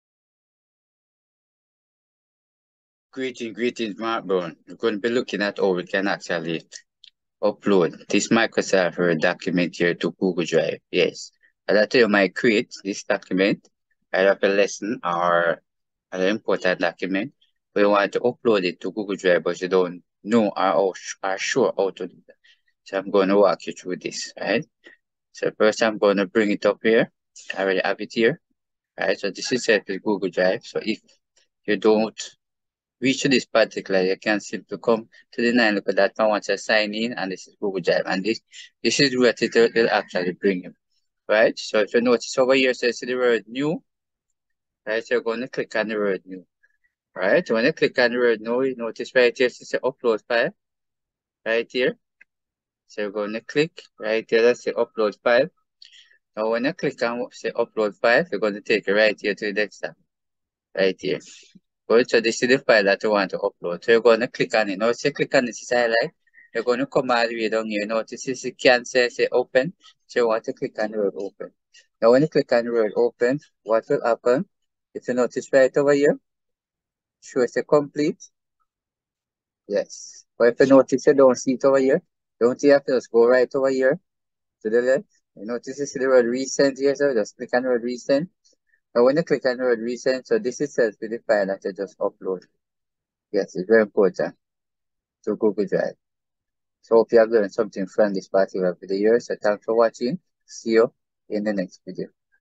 HOW TO UPLOAD FILE TO GOOGLE DRIVE
In this step-by-step video, you will learn how upload file to google drive. Google drive is a great place to store all your digital files to enable you to access them from any device. This short guide will show you how to move your files  from your device  onto your My Drive.